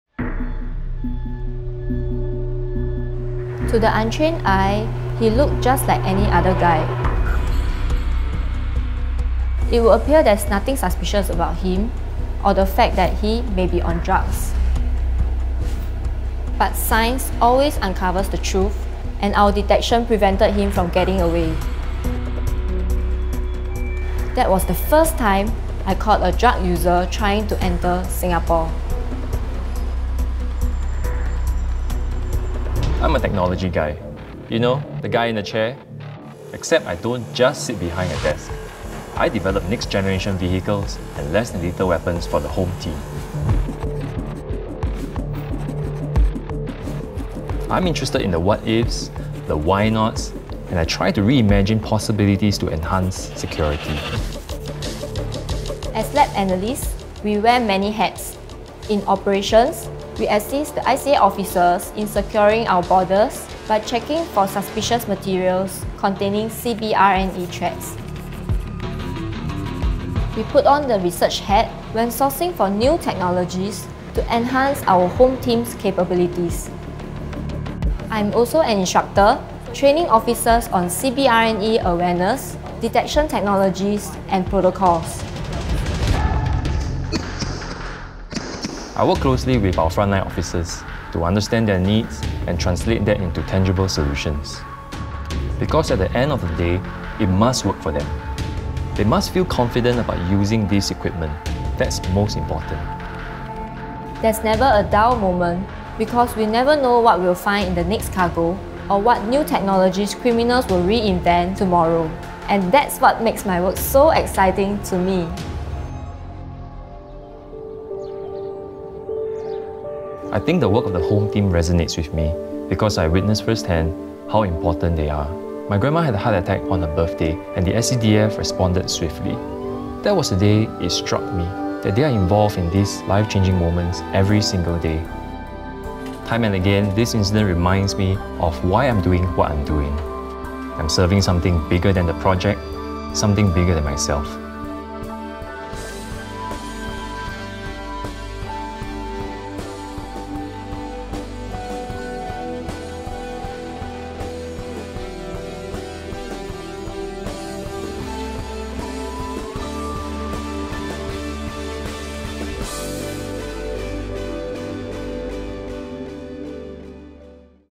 HTX Scholarship
The HTX Scholarship is for outstanding STEM students who are passionate about advancing Science and Technology to innovate solutions for our Home Team's operations so as to eXponentially impact the safety and security of Singapore.